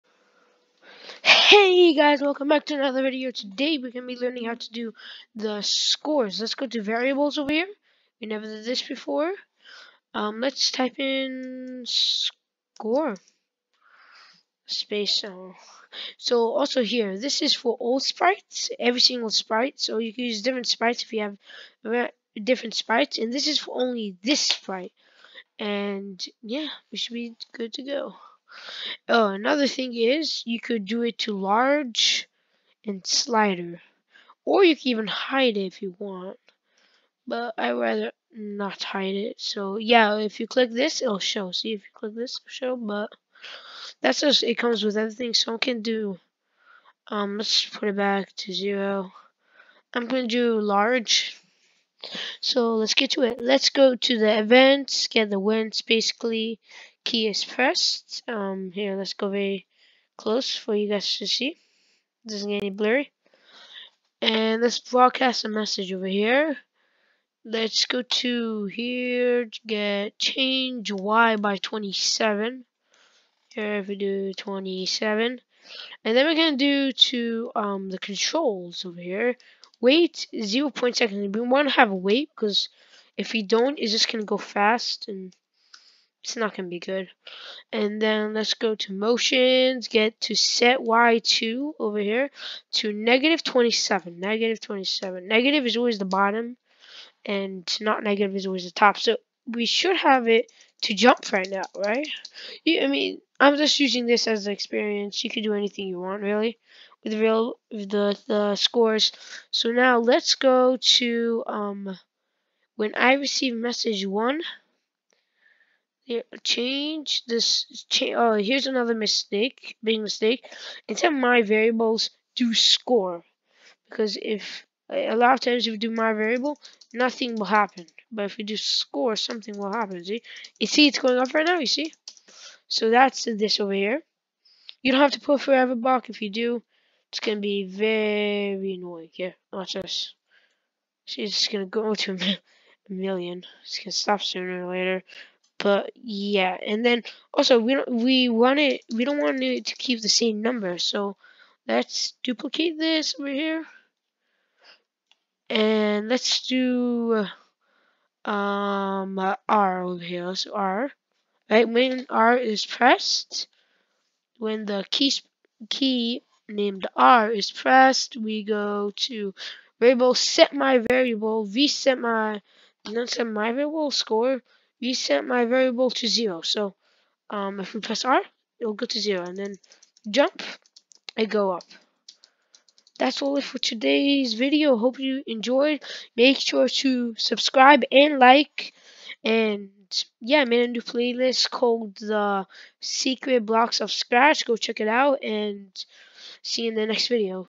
How to make a score in scratch and how to use variables.
How to make a score in scratch and variables part of the how to use blocks in scratch playlist.